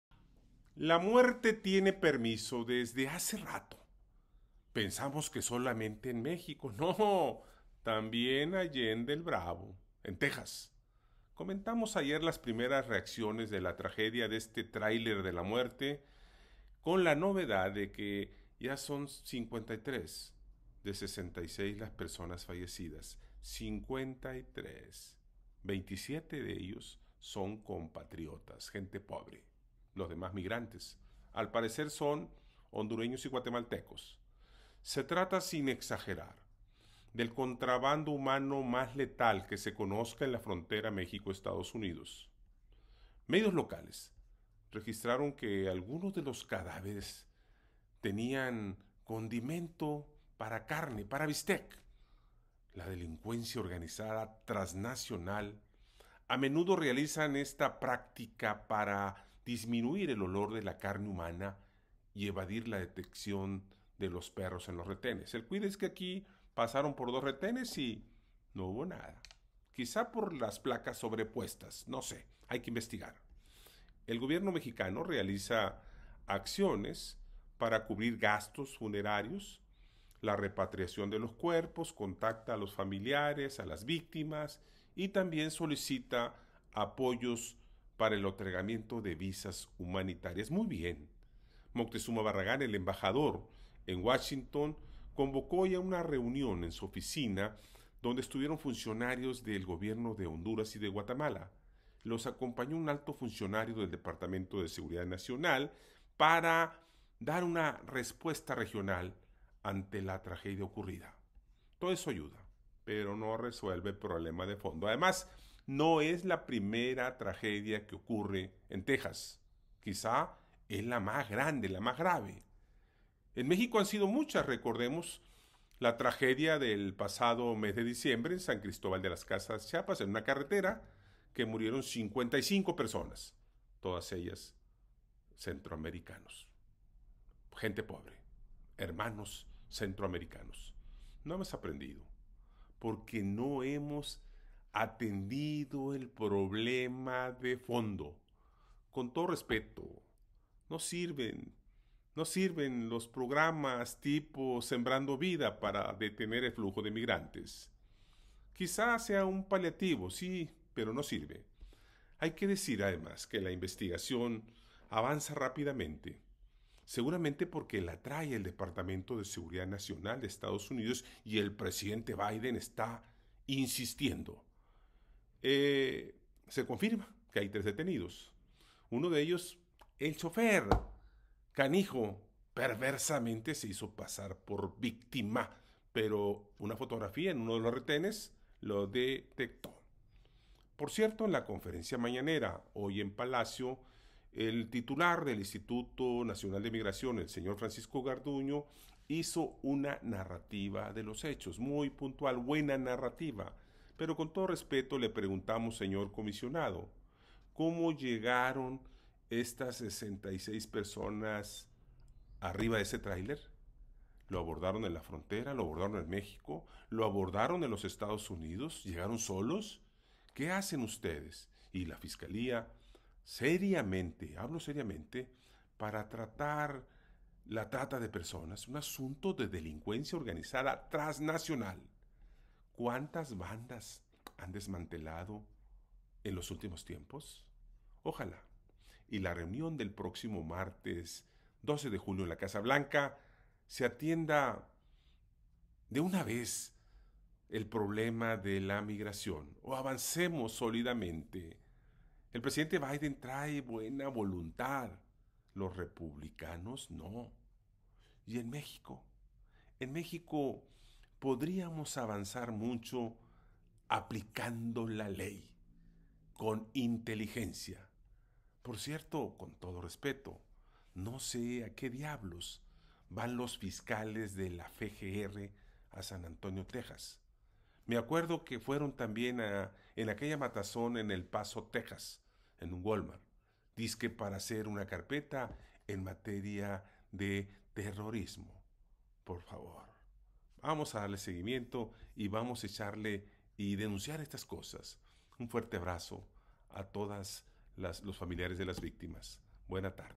El tráiler de la muerte en San Antonio, Texas;
La muerte tiene permiso desde hace rato, y no sólo en México sino también allende el Bravo, en Texas.
Comentamos ya las primeras reacciones de la tragedia del tráiler de la muerte en San Antonio, Texas...
Con la novedad de que ya son 53 de 66 las personas fallecidas, 27 son compatriotas, los demás migrantes al parecer guatemaltecos y hondureños; se trata sin exagerar del contrabando humano más letal que se conozca en la frontera México-EU.; 27 de los 53 difuntos están identificados.
Medios locales registran que algunos de los cadáveres tenían condimento para bistec; la delincuencia organizada trasnacional, a menudo realizan esta práctica para disminuir el olor de la carga humana y evadir la detección de los perros en los retenes.
El gobierno de México realiza acciones para cubrir los gastos funerarios, la repatriación de los cuerpos, contacta a los familiares y también solicita apoyos para el otorgamiento de visas humanitaria.
Esteban Moctezuma Barragán convocó a una reunión en su oficina en Washington DC donde estuvieron funcionarios de los gobiernos de Guatemala y Honduras, los acompañó un funcionario de DSH, Blas Núñez Neto para coordinar "una respuesta regional ante la tragedia ocurrida en San Antonio, Texas.
Todo eso ayuda, pero no resuelve el problema de fondo. No es la primera tragedia que ocurre en EU, ni la primera en México; recordemos la del pasado diciembre en una carretera en San Cristóbal de las Casas, Chiapas que murieron 55 personas y más de 100 heridos, todos ellos hermanos centroamericanos, pobres... No hemos aprendido porque no henos atendido el problema de fondo, con todo respeto no sirven para mucho los programas "Sembrando Vida": nada de eso detiene el flujo de migrantes...
Pero hay que decir que investigación avanza rápido, seguramente porque la trae el Departamento de Seguridad nacional de EU; se confirma hay tres detenidos, uno de ellos es el chofer que perversamente se hizo pasar por víctima, él solo es un, pero de los verdaderos criminales, la Delincuencia Organizada Trasnacional...
Por cierto, en conferencia en Palacio Nacional el titular del INM, Francisco Garduño hizo una narrativa de los hechos y dijo que el tráiler pasó por dos retenes de la Oficina de Aduanas y Protección Fronteriza y no fue revisado. Buena narrativa del señor comisionado...
Le preguntamos ¿cómo llegaron estas 66 personas a ese tráiler?
¿Lo abordaron en la frontera? ¿Llegaron solos? ¿Qué hace el INM y la FGR seriamente contra la trata de personas? ¿Cuántos bandas de han desmantelado en este gobierno? Ojalá y en la reunión del próximo martes 12 de julio en la Casa Blanca se atienda de una vez por todas el problema de la migración, o avancemos; Biden trae buena voluntad, los republicanos no...
¿Y en México?
Podríamos avanzar mucho aplicando la Ley... Por cierto, no sé a qué van los fiscales de México a San Antonio...
PD. El cartón que ilustra es de Rictus de El Heraldo.